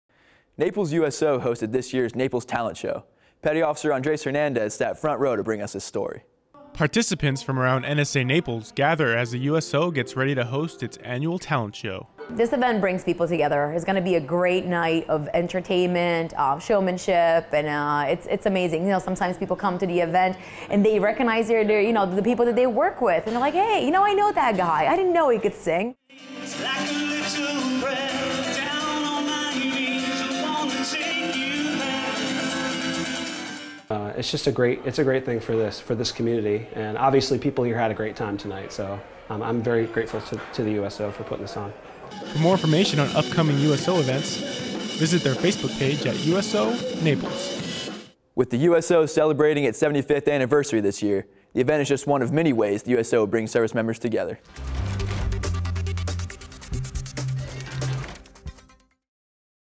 Naples Got Talent
USO Naples Hosts Their Annual Talent Show.